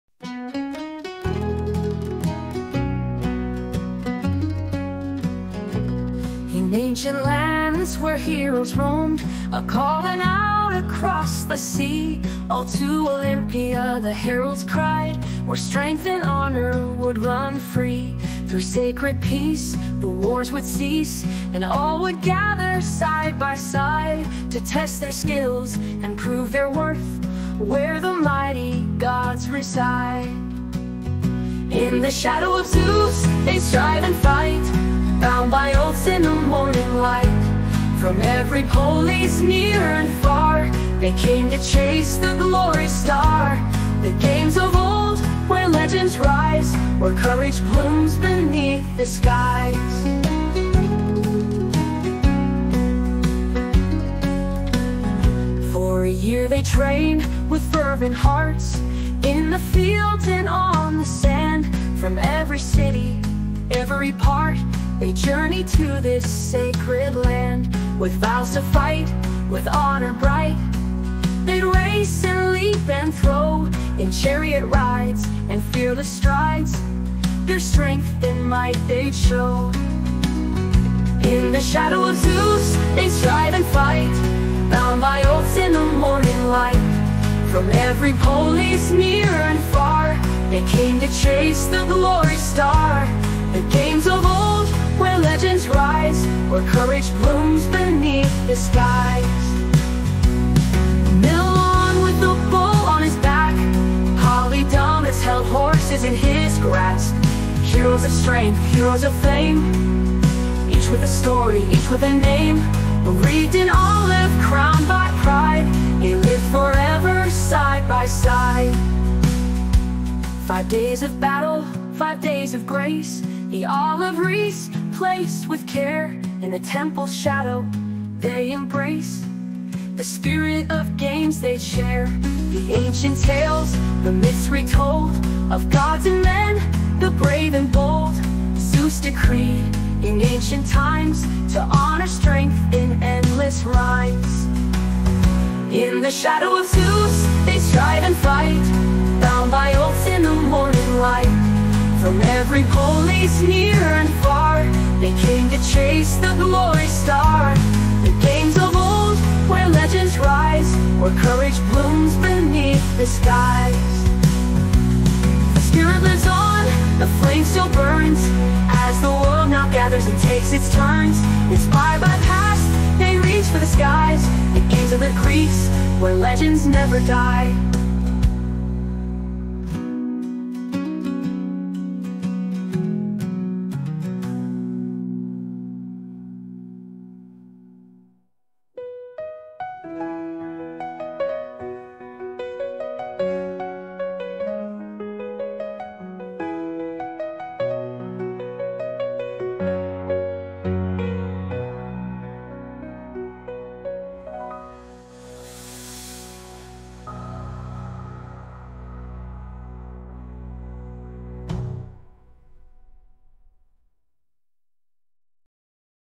The Ancient Olympic Games. History in songs.
### **The Ancient Olympic Games**

**1. A Festival Uniting the Greeks**
In ancient Greece, the Olympic Games were a significant event that united all the Greek city-states, despite their frequent conflicts. In the year of the Games, heralds would proclaim: "All to Olympia! Sacred peace is declared; the roads are safe! Let the strongest win!" The Games were held every four years in Olympia, a small town in southern Greece. During the Games, all wars and battles were forbidden, allowing participants and spectators from all over Hellas to travel safely to the event. The Games began in 776 BC and were held regularly from then on.

**2. Preparation for the Games**
All free Greeks could participate in the competitions, regardless of their wealth or noble status, but women were not allowed, even as spectators, since the Olympic Games were dedicated to Zeus and considered an exclusively male festival. Athletes began training a year before the Games, and a month prior, they arrived in southern Greece to continue their preparation near Olympia. These long periods of training were feasible only for the wealthy, as the poor could not afford to stop working for such extended periods.

**3. The Five Days of the Olympic Games**
The Olympic Games lasted five days. On the first day, athletes made sacrifices to the gods and swore an oath of fair competition, while the judges pledged to make just decisions. The next three days were dedicated to sporting events in the stadium. One of the main competitions was the pentathlon, which included running, long jump, javelin throw, discus throw, and wrestling. There were also boxing matches, races in armor, and chariot races. Chariot races, held at the hippodrome, were a thrilling spectacle but also very dangerous, with frequent collisions and accidents that caused injuries to the drivers.

**4. Rewards and Glory for the Victors**
On the final day of the Games, the victors were awarded olive wreaths in front of the temple of Zeus. The chief judge placed the wreaths on the winners' heads, and a herald announced their names and hometowns. Victors became heroes in their cities, greeted with honors, and their statues were erected. Some cities even granted them free meals for life.

**5. Legendary Athletes and Their Feats**
Among the legendary athletes, Milon holds a special place, having won the Olympic Games six times in a row. As a young man, he lifted a calf every day, and when it grew into a bull, Milon continued to carry it on his shoulders. Polydamas, another famous athlete, was known for holding a team of four horses with one hand and once strangled a lion like the hero Heracles. The runner and boxer Theagenes was said to have carried a bronze statue he fancied from the marketplace, astonishing everyone with his strength.

**6. The Myth of the Founding of the Olympic Games**
The ancient Greeks believed that the Olympic Games were established in honor of Zeus's victory over his father, Cronus, who devoured his children out of fear of losing his power. When Zeus grew up, he defeated Cronus and freed his brothers and sisters. In memory of this victory, Zeus decreed athletic competitions to be held in Olympia.

**Conclusion**
The Olympic Games became an essential part of Greek culture and a symbol of Greek unity. They brought together people from different city-states, inspired them to achieve great feats, and stood as a symbol of fair competition and peace. The Olympic traditions have survived through the centuries and continue to inspire people to this day.